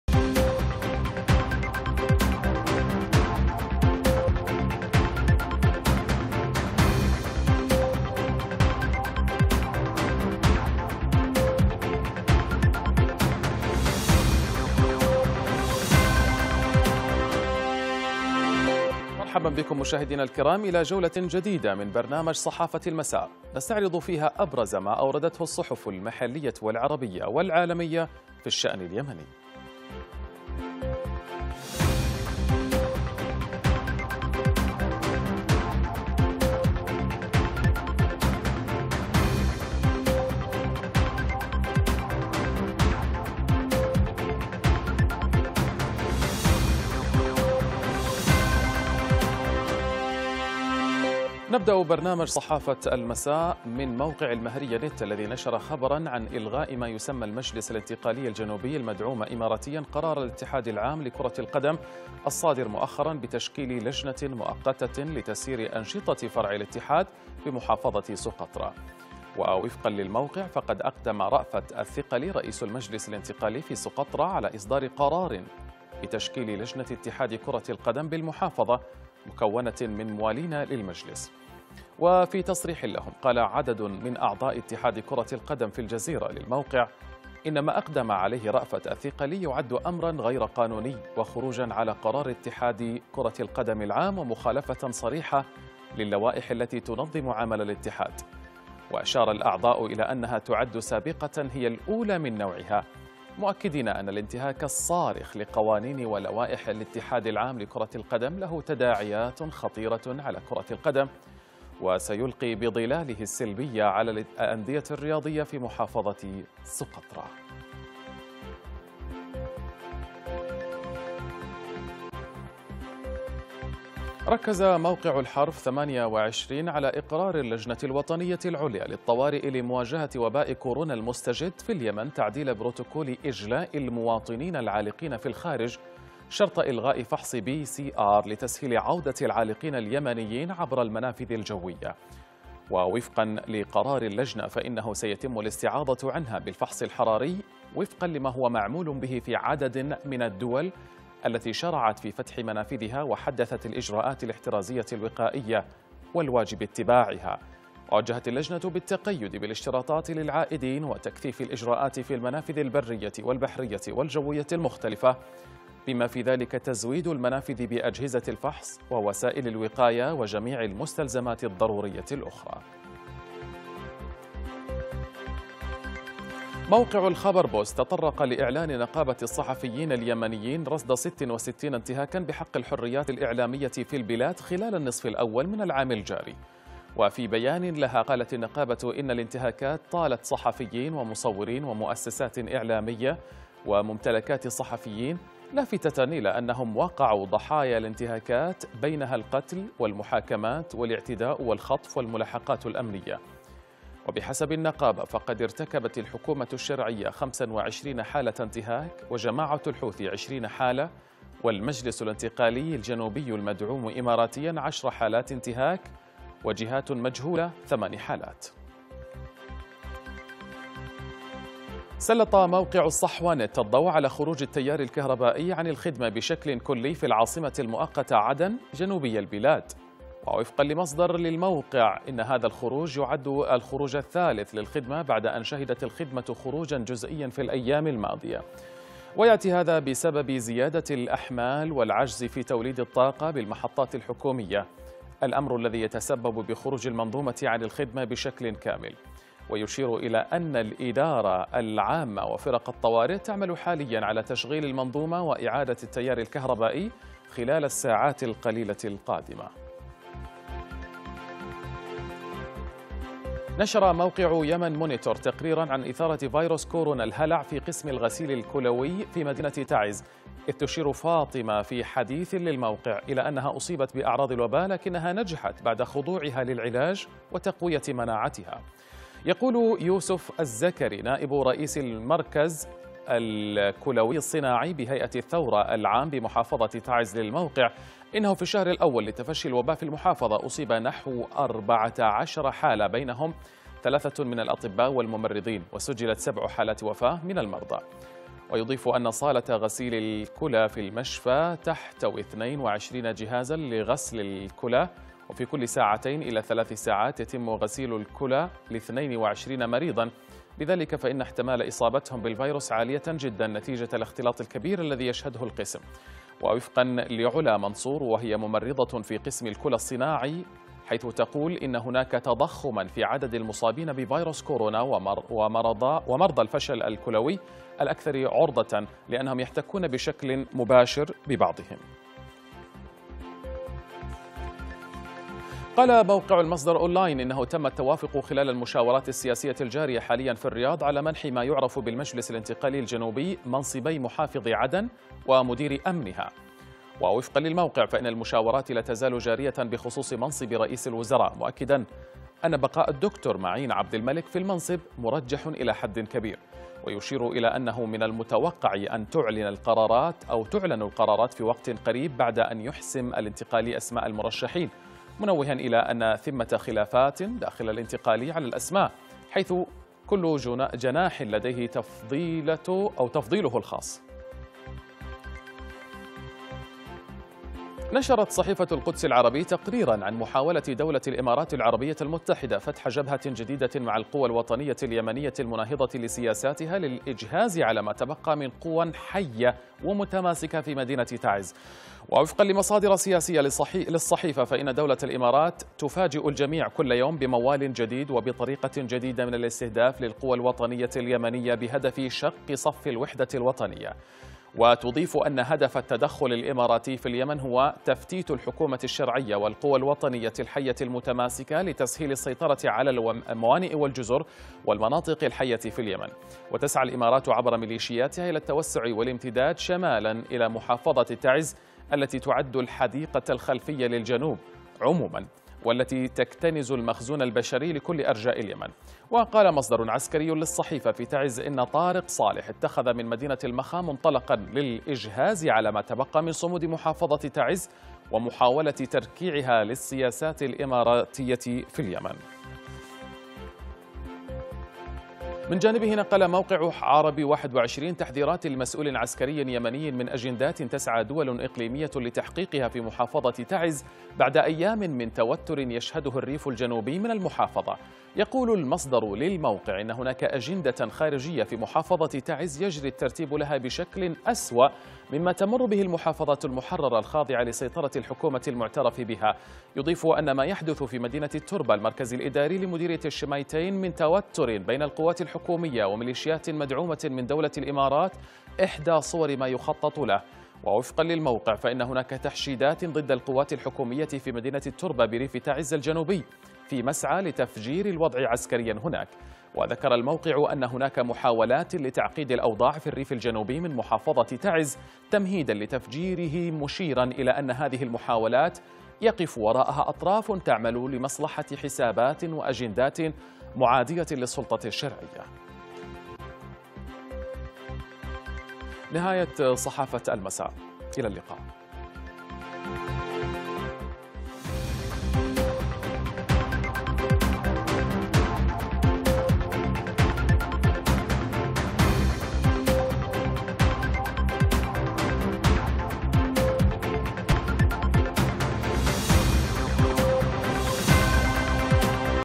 الإمارات تستهدف تعز للإجهاز على ما تبقى من تماسك القوى الوطنية المناهضة لسياساتها في اليمن
- المهرية نت : سقطرى.. مليشيا الانتقالي تنقلب على النظام الأساسي لاتحاد كرة القدم
- الحرف28 : الحكومة اليمنية تقر تعديل بروتوكول اجلاء العالقين في الخارج
- الخبر بوست : اليمن ..تسجيل 66 انتهاكا للحريات الإعلامية خلال النصف الأول من العام الجاري بينها حالتي قتل
- الصحوة نت : كهرباء عدن تخرج عن الخدمة !
- يمن مونيتور : كورونا يثير الهلع في قسم “الغسيل الكلوي” في مدينة تعز وسط اليمن
- المصدر أون لاين : الاتفاق على أن يكون محافظ ومدير أمن عدن من "الانتقالي" وهؤلاء أبرز المرشحين لمنصب المحافظ
- القدس العربي : الإمارات تستهدف مدينة تعز للإجهاز على ما تبقى من تماسك القوى الوطنية المناهضة لسياساتها
- عربي21 : مسؤول عسكري يحذر من أجندة مشبوهة بمحافظة تعز